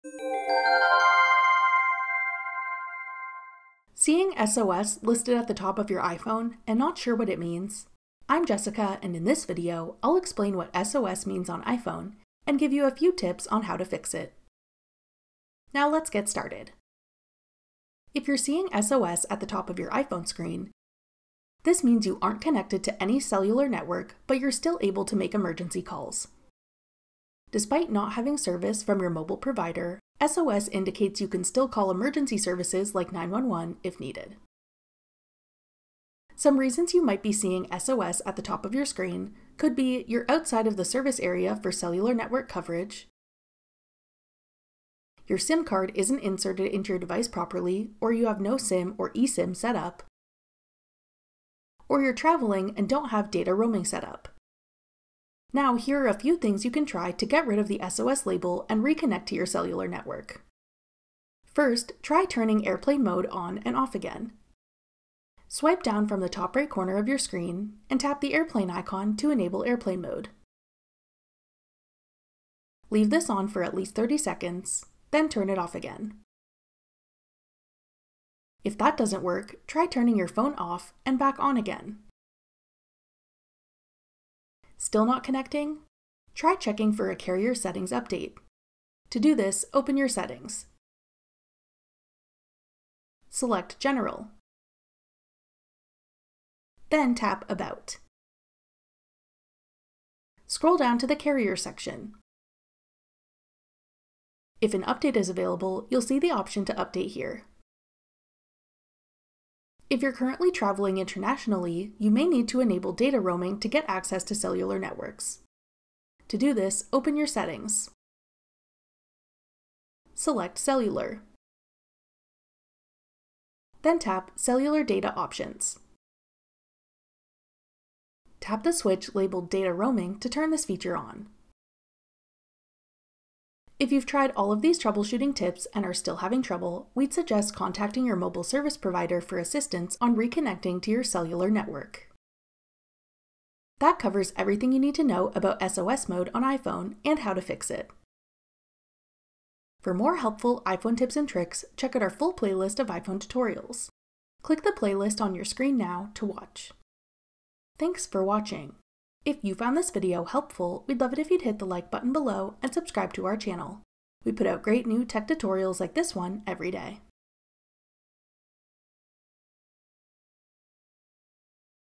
What Does SOS Mean on iPhone? + Troubleshooting Tips
Seeing “SOS” listed at the top of your iPhone, and not sure what it means? Watch this video to learn what SOS means on iPhone, and a few tips on how to fix it!

CHAPTERS:
0:00 Intro
0:16 What does SOS mean on iPhone?
0:36 Why does your iPhone say SOS?
1:01 Tip 1: Turn airplane mode off & on
1:18 Tip 2: Restart your iPhone
1:24 Tip 3: Check for carrier settings update
1:49 Tip 4: Enable data roaming
2:13 Tip 5: Contact your mobile carrier
2:32 Get more iPhone tips